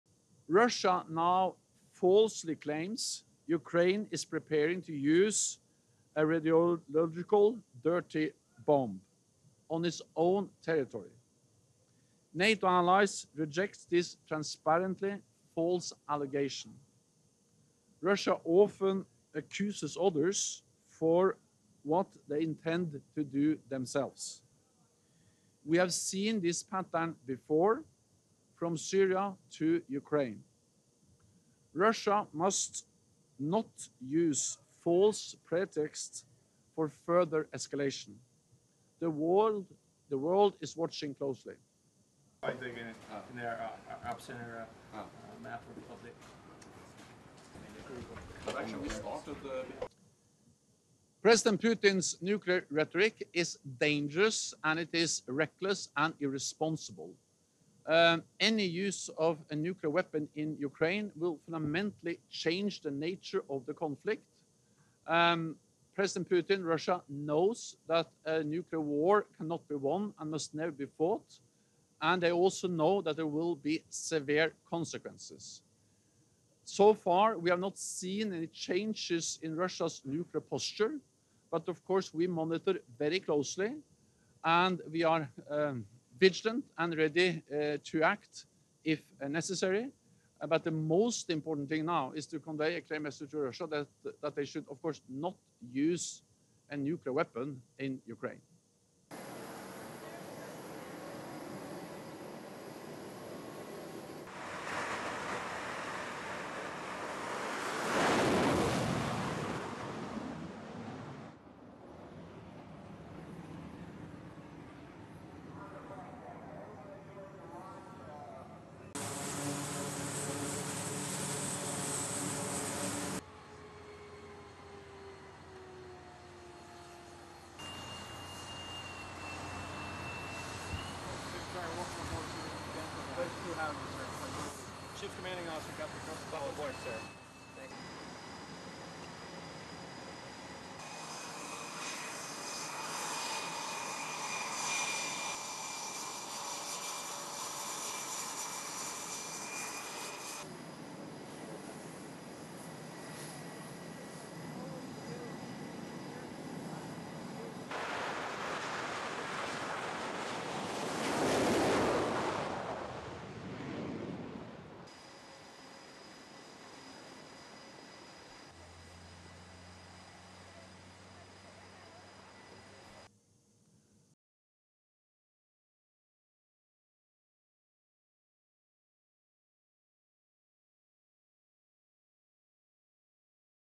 Russian 'dirty bomb' allegations are 'transparently false' says Stoltenberg
Russian allegations that Ukraine is preparing to use a "dirty bomb" are "transparently false", NATO Secretary General Jens Stoltenberg said on Tuesday (October 25).

The head of the military alliance made the comments on a trip to a U.S. aircraft carrier stationed in the Adriatic Sea, and before the U.N. Security Council meeting at which Russia raised the issue.

"NATO allies reject this transparently false allegation," Stoltenberg told a news conference on board the USS George H. W. Bush.

"Russia often accuses others for what they intend to do themselves. We have seen this pattern before from Syria to Ukraine. Russia must not use false pretexts for further escalation," he added.

The allegations follow hints from Moscow that it might be forced to use a tactical nuclear weapon against Ukraine. Ukrainian President Volodymyr Zelenskiy said the dirty bomb allegation showed Russia was planning such an attack and seeking to blame Kyiv.

Stoltenberg echoed comments from the United States warning Russia of "severe consequences" for any use of nuclear weapons.

The visit to the ship coincided with a NATO deployment called "Neptune Strike" which the alliance describes as a "peacetime vigilance" mission including more than 80 aircraft, 14 ships and some 6,000 personnel.

Recently, U.S. Navy F18s had flown from the ship to Lithuania and joined Canadian aircraft for patrols over Romania, Stoltenberg said.